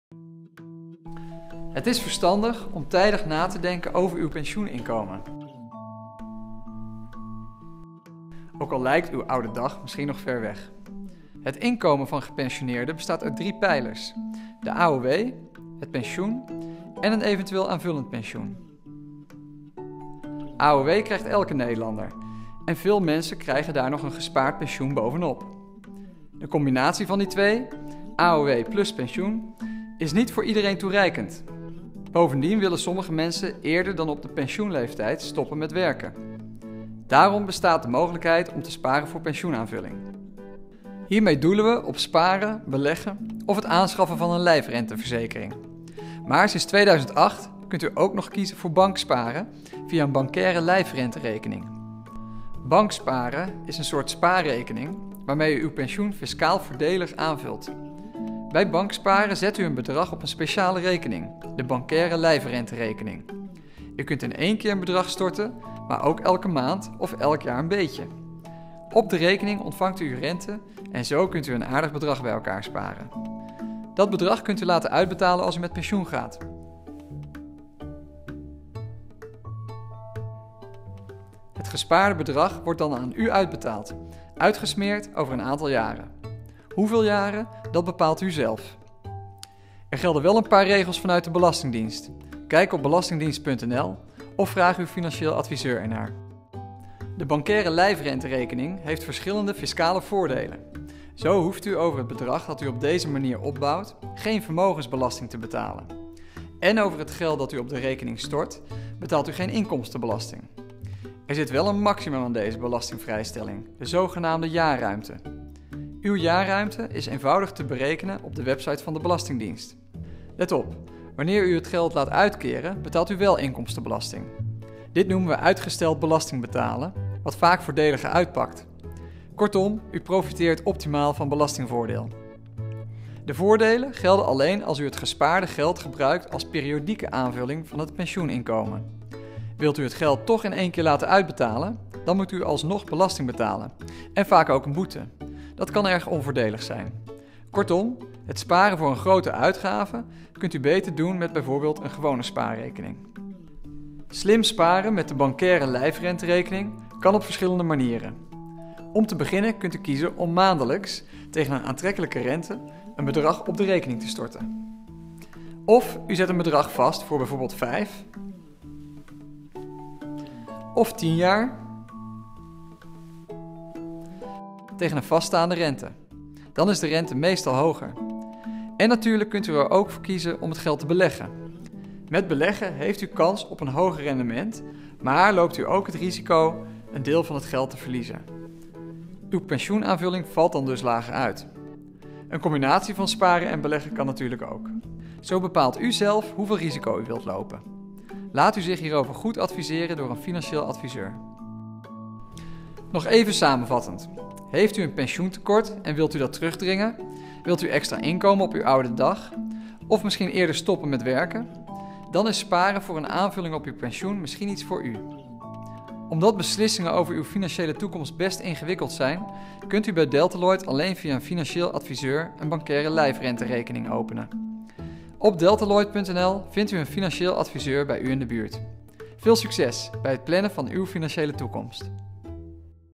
Delta Lloyd | Lijfrenterekening
Het is verstandig om tijdig na te denken over uw pensioeninkomen. Namens Delta Lloyd maakte Yune een video waarin uitleg wordt gegeven over een bancaire lijfrenterekening: sparen voor een aanvulling op uw pensioen.